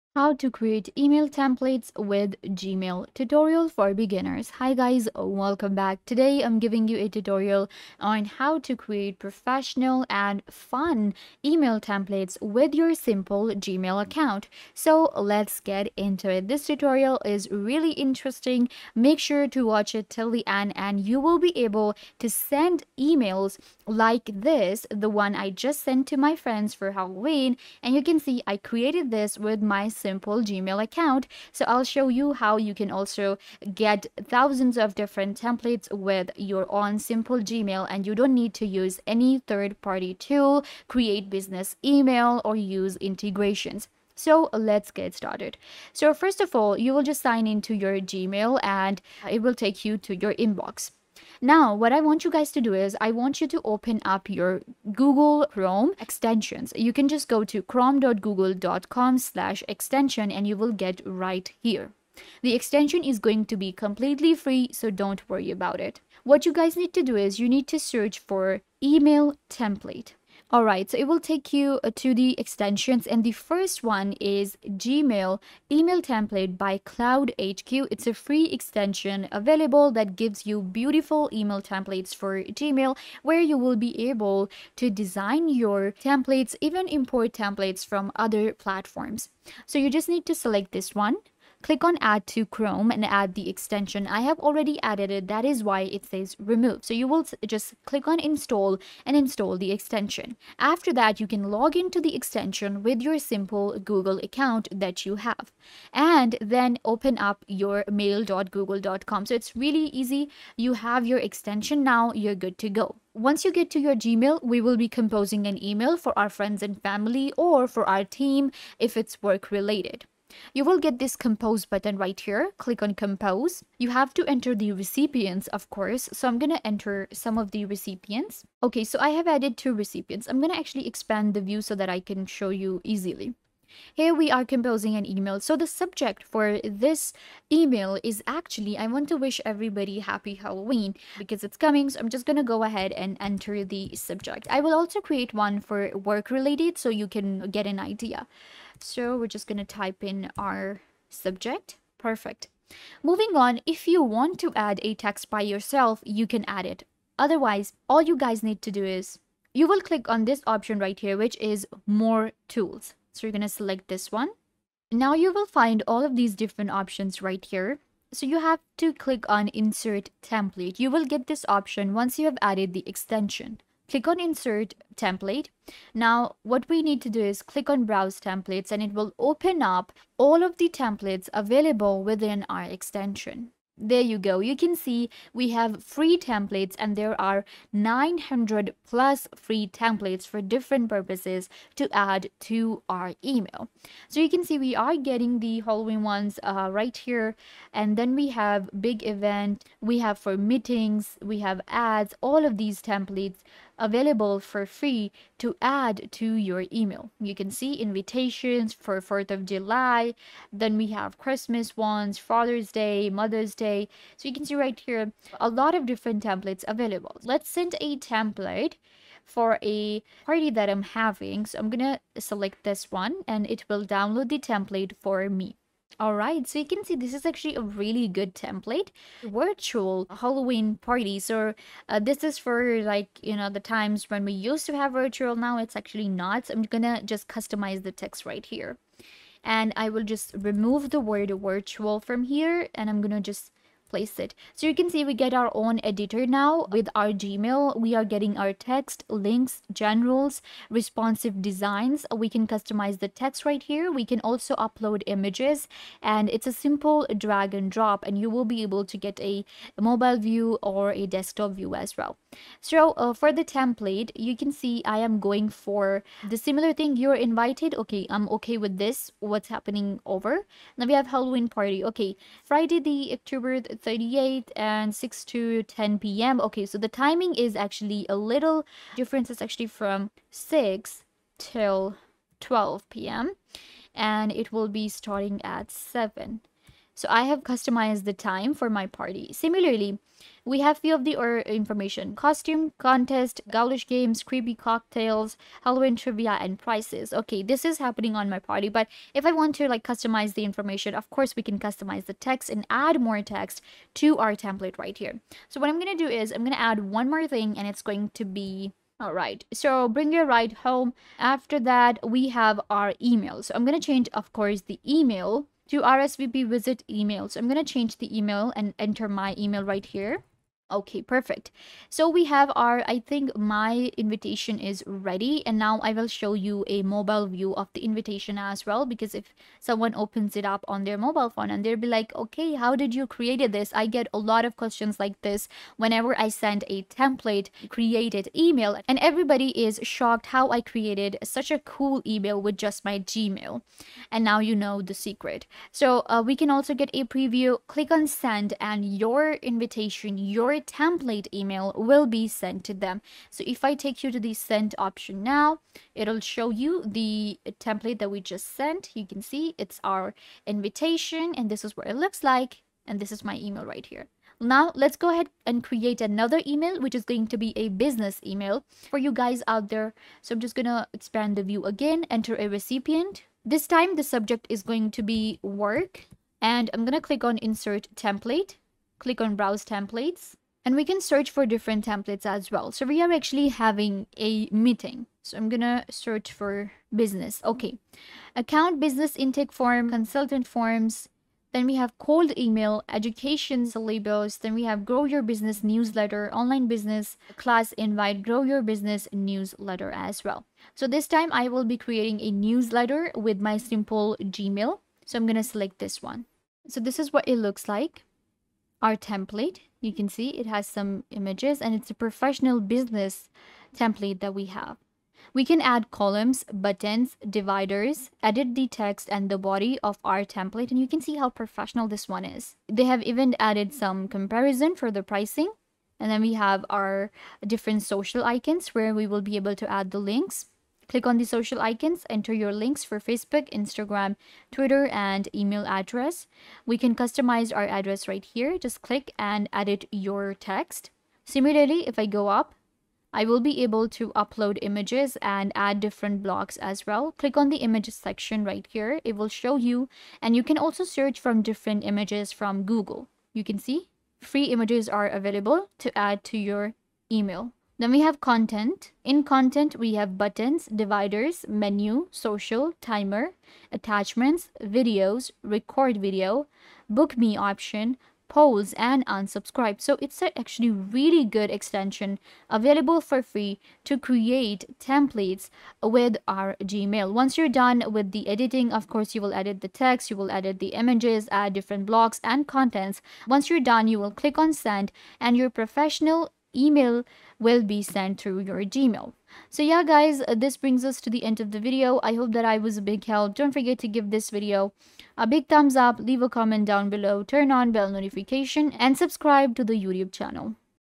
How To Create Email Templates With Gmail (2025) | Tutorial For Beginners
In this video, we show you step-by-step how to create email templates with Gmail. This tutorial makes it easy to save time, reuse emails, and streamline your communication — even if you’re a beginner.